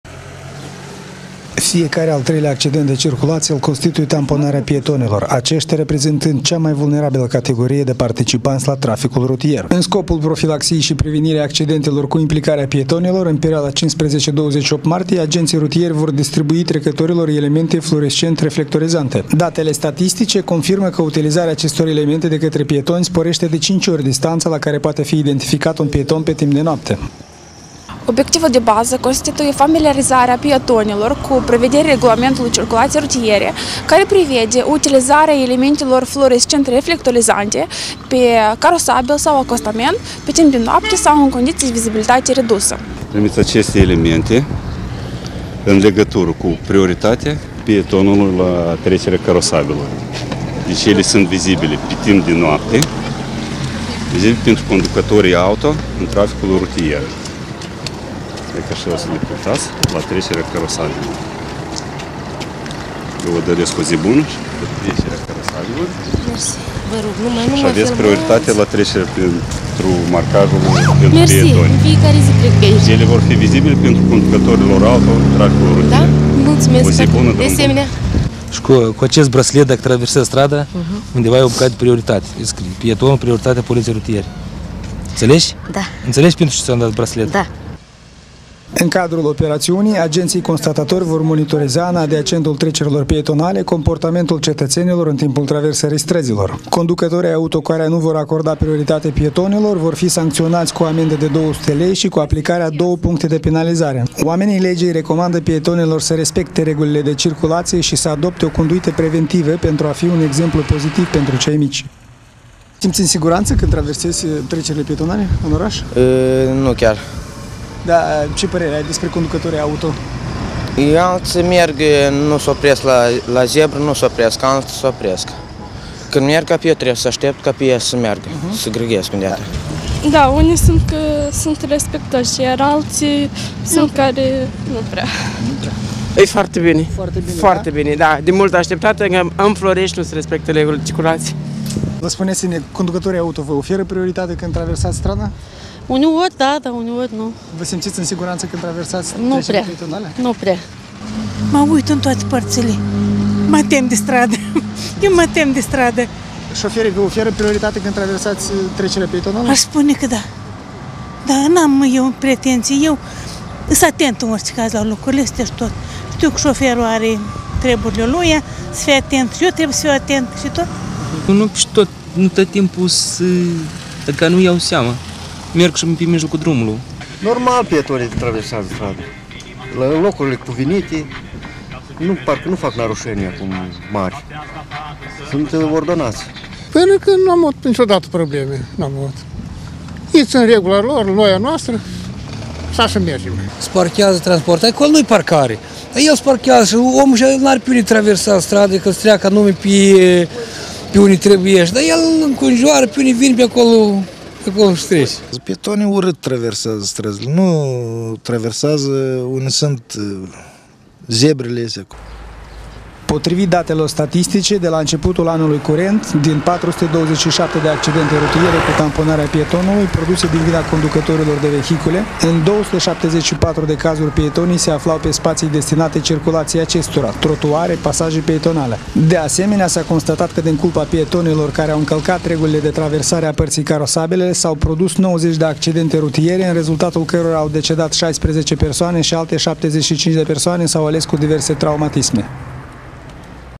Poliţia Rutieră în campanie de securizare a pietonilor
Fiecare al treilea accident de circulaţie îl constituie tamponarea pietonilor, aceştia reprezentând cea mai vulnerabilă categorie de participanţi la traficul rutier.
În scopul profilaxiei şi prevenirii accidentelor cu implicarea pietonilor, în perioada 15 - 28 martie agenții rutieri vor distribui trecătorilor elemente fluorescent-reflectorizante. Datele statistice confirmă că utilizarea acestor elemente de către pietoni sporeşte de 5 ori distanţa la care poate fi identificat un pieton, pe timp de noapte.
În cadrul operaţiunii, agenţii constatatori vor monitoriza, în adiacentul trecerilor pietonale, comportamentul cetăţenilor în timpul traversării străzilor.
Conducătorii auto, care nu vor acorda prioritate pietonilor, vor fi sancţionaţi cu o amenda de 200 lei şi cu aplicarea a 2 puncte de penalizare.
Oamenii legii le recomandă pietonilor să respecte regulile de circulaţie şi să adopte o conduită preventivă pentru a fi un exemplu pozitiv pentru cei mici.
Potrivit datelor statistice, de la începutul anului curent, din 427 de accidente rutiere cu tamponarea pietonului, produse din vina conducătorilor de vehicule, în 274 de cazuri pietonii se aflau pe spaţiile destinate circulaţiei acestora (trotuare, pasaje pietonale).
De asemenea, s-a constatat că din culpa pietonilor, care au încălcat regulile de traversare a părţii carosabile, s-au produs 90 de accidente rutiere, în rezultatul cărora au decedat 16 persoane şi altele 75 de persoane sau ales cu diverse traumatisme.